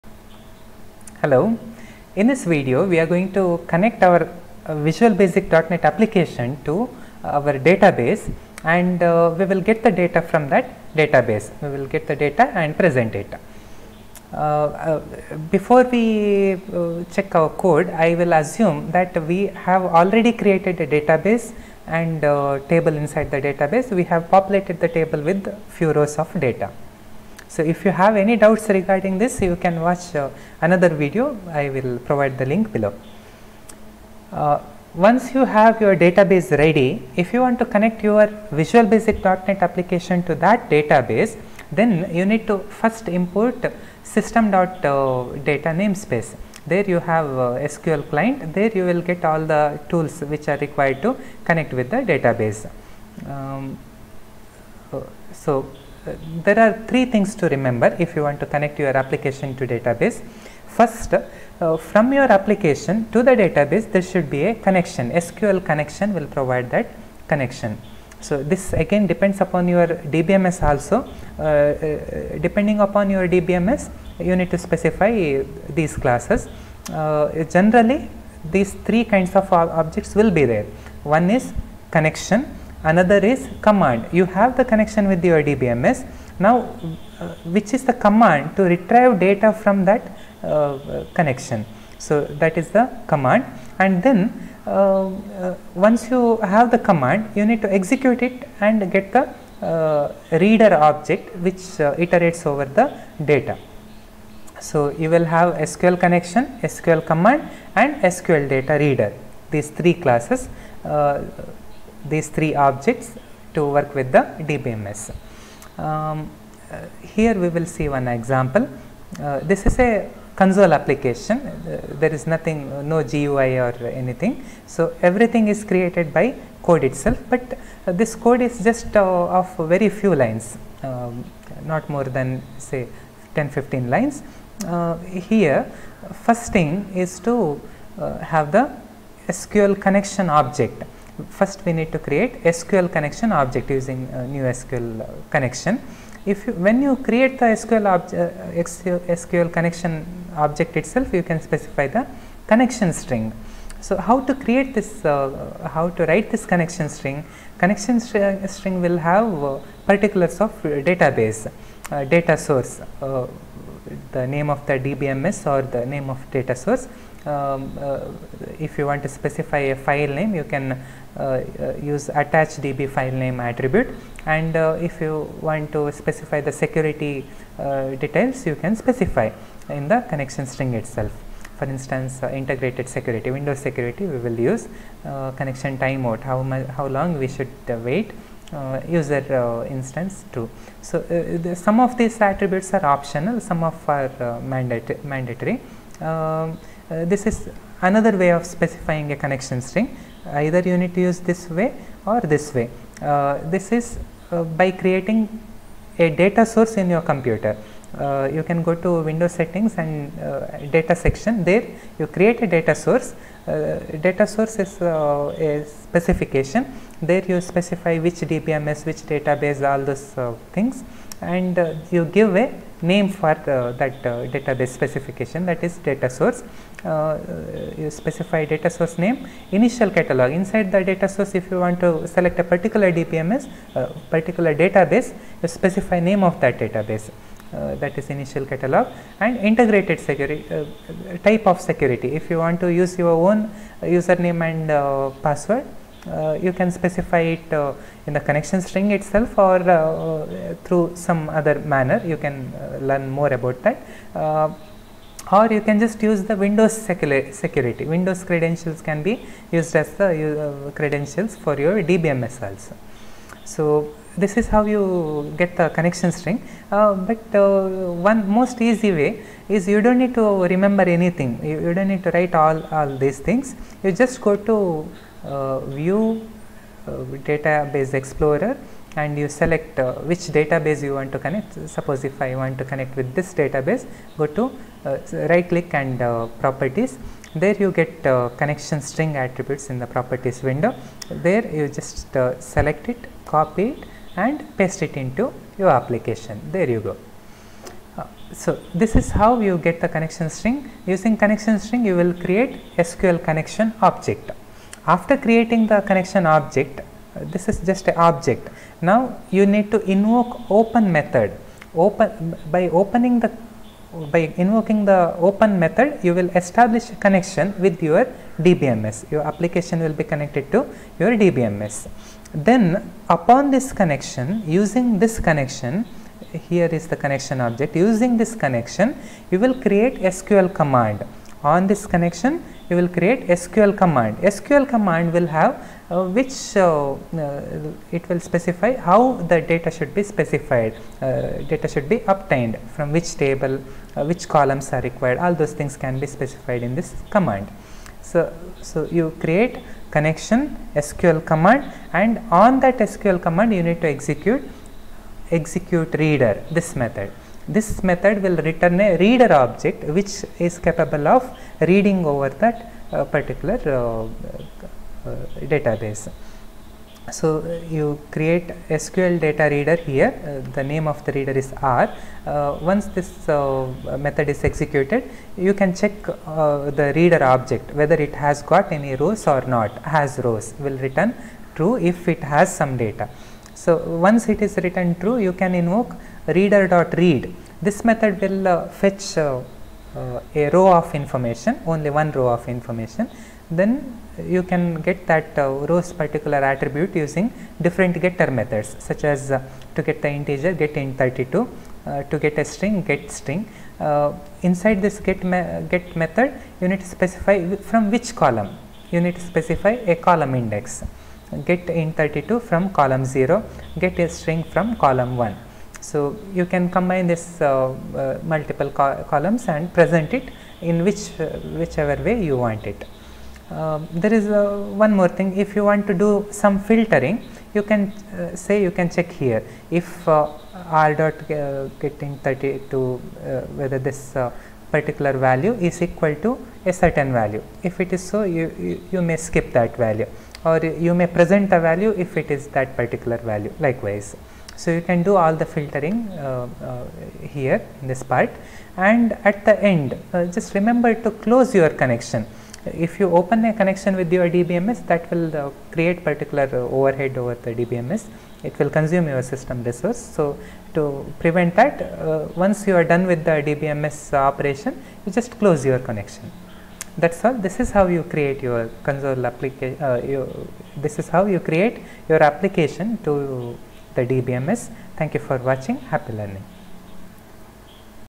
02 Connecting Your Visual Basic .NET Console Application to Database and Retrieving Data
We will connect a Visual Basic .NET console application to a MS SQL Express database and read the data from a table.